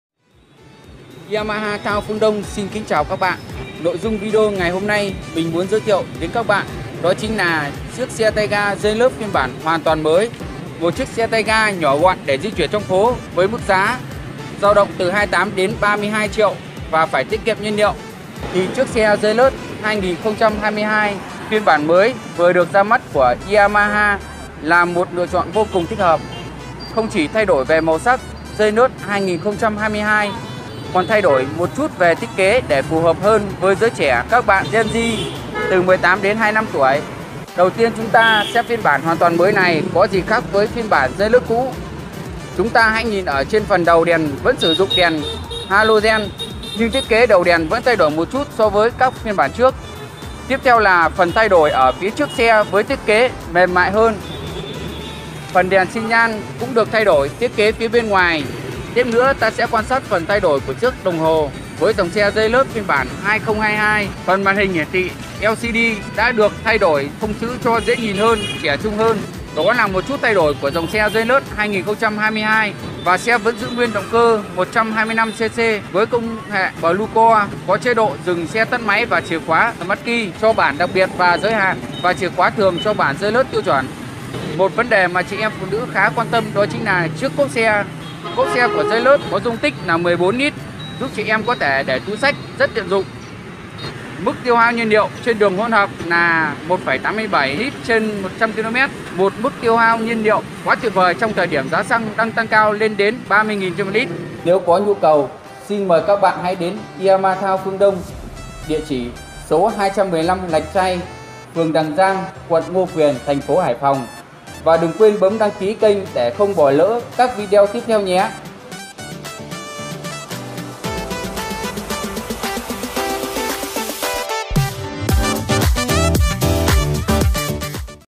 Review xe tay ga tiết kiệm xăng Yamaha Janus 125 HOÀN TOÀN MỚI
Phương Đông Motor hiện đang kinh doanh cả 3 phiên bản của Janus 2022 (đầy đủ màu sắc cho khách hàng lựa chọn).

Xe Janus có thiết kế trẻ trung cùng khả năng vận hành mượt mà với mức giá rất phải chăng. Sở hữu động cơ Blue Core, hệ thống Stop & Start và chức năng One push start, xe Janus có khả năng tiết kiệm nhiên liệu với mức tiêu thụ chỉ 1,87 lít/100km - nằm trong Top đầu xe tay ga tiết kiệm xăng nhất Việt Nam.

Xe đang có sẵn hàng tại Phương Đông Motor :
🏬 Địa chỉ : 215 Lạch Tray, Đằng Giang, Ngô Quyền, Hải Phòng

📎 Phương Đông Motor (Yamaha Town Phương Đông - Đại lý ủy quyền chính thức của Yamaha Motor Việt Nam)

© Bản quyền thuộc về Phương Đông Motor
© Copyright by Phuong Dong Motor channel ☞ Do not Reup